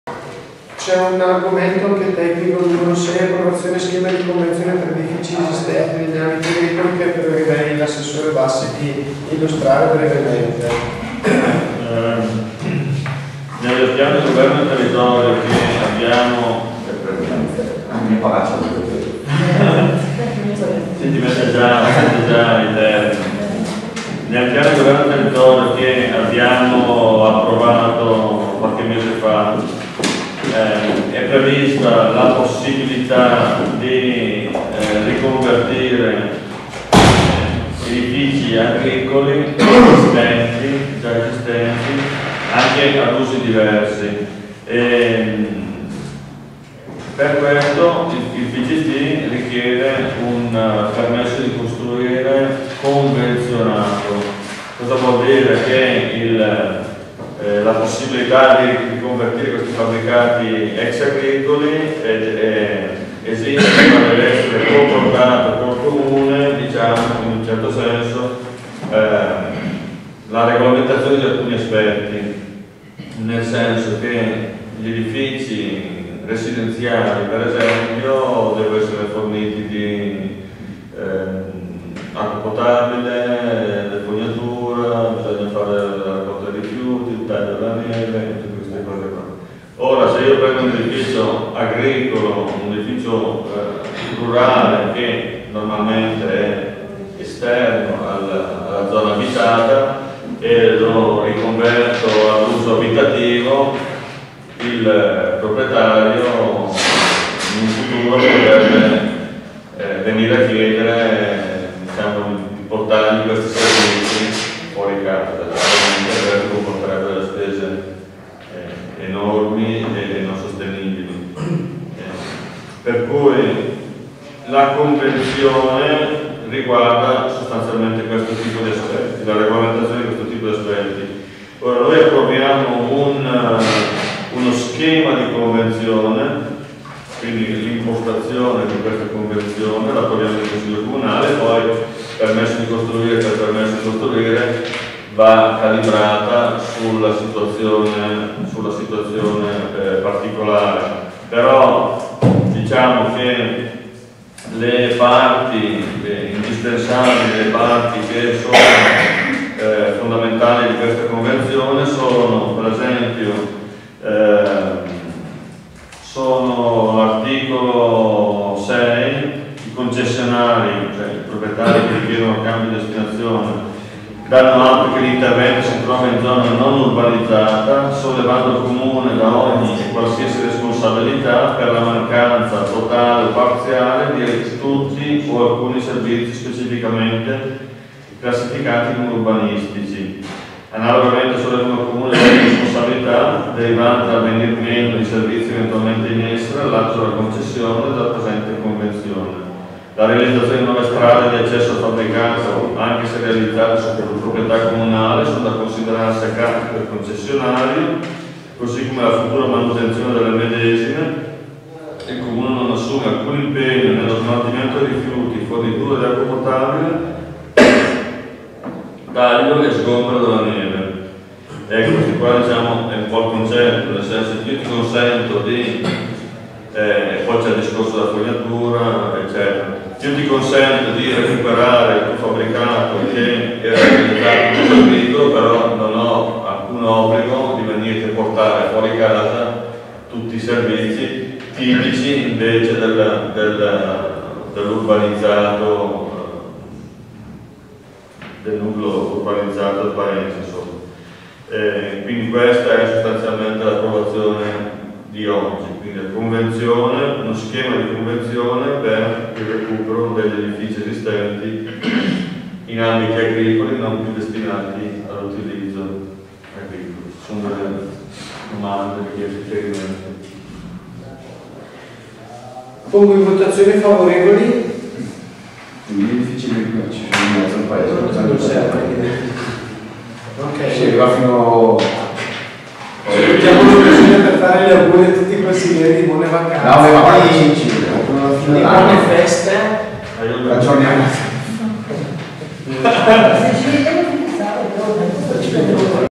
CC 30-11-2013 Punto 6: Approvazione schema di convenzione per edifici esistenti...
Registrazione del Consiglio Comunale di Ossimo (BS) del 30/11/2013. Discussione sul punto all'ordine del giorno 6: Approvazione schema di convenzione per edifici esistenti negli ambiti agricoli.